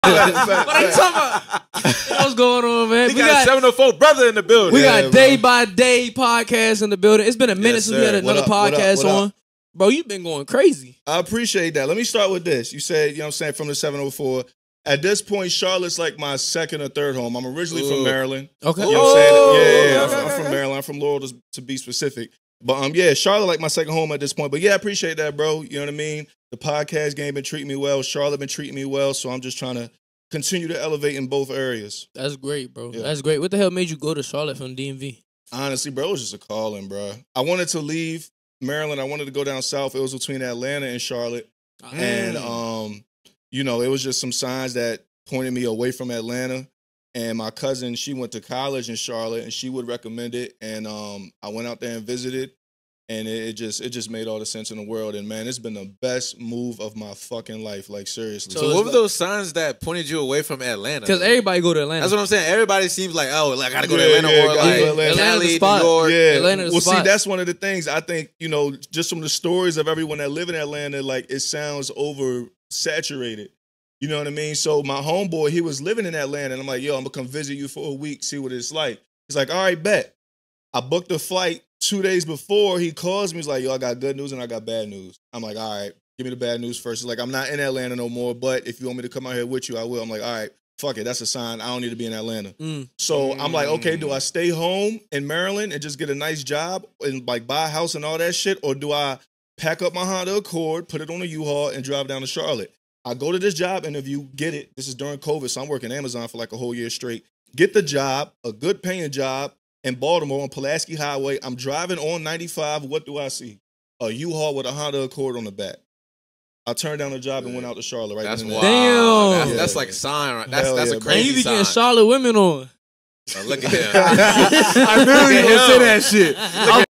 Why I Moved To Charlotte Over Atlanta | A Sign From God
While being featured on  @nothingbutdiscussion  podcast, Dai explains what led him to move to Charlotte from the DMV over Atlanta.